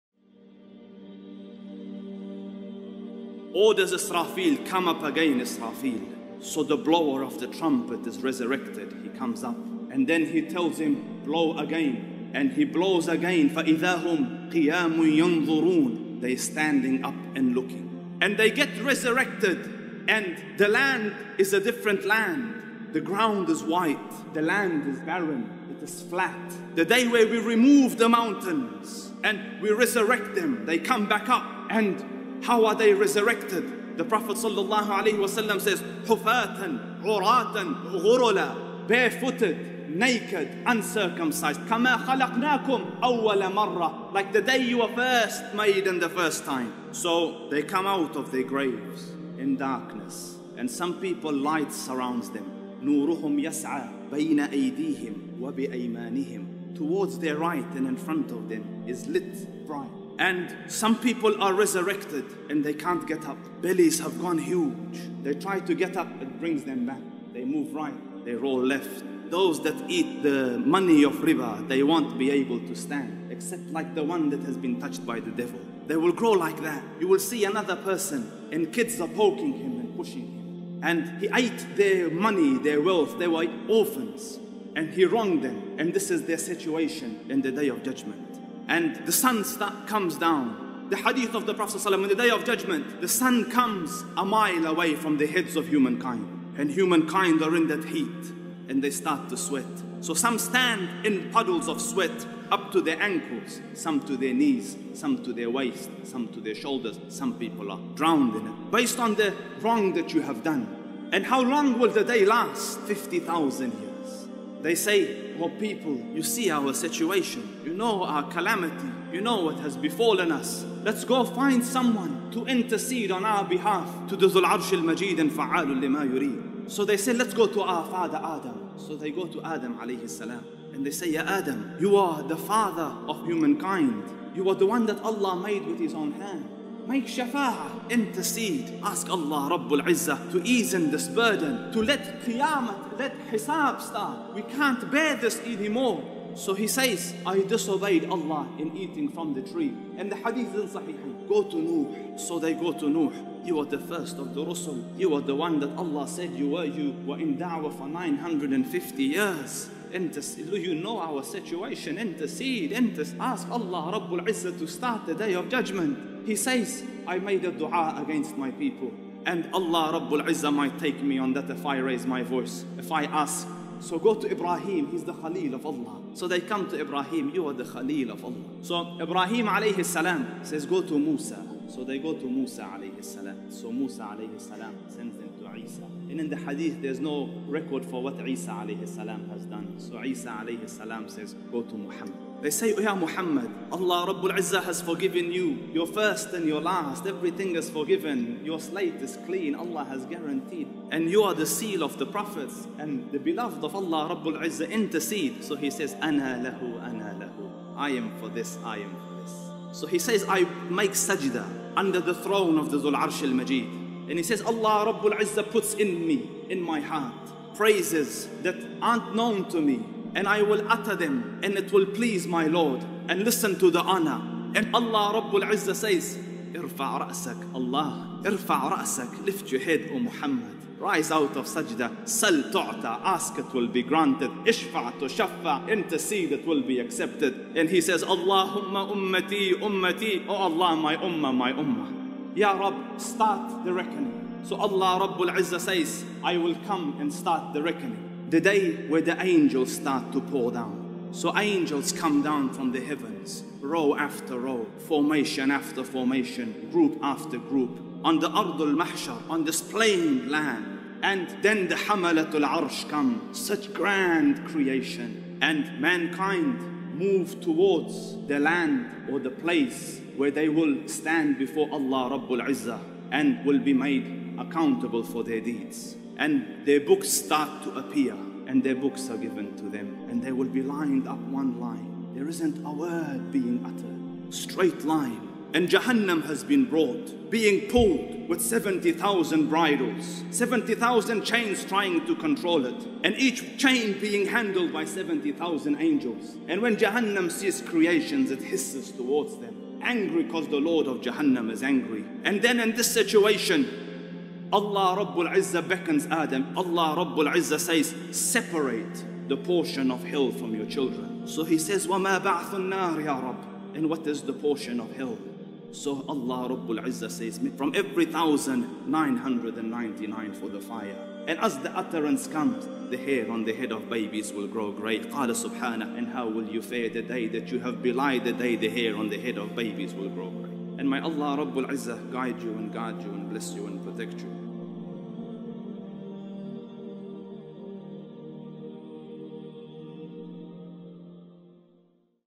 ALLAH ORDERS ANGEL ISRAFEEL TO BLOW THE TRUMPET & JUDGEMENT DAY BEGINS!
Speaker: Ustadh Wahaj Tarin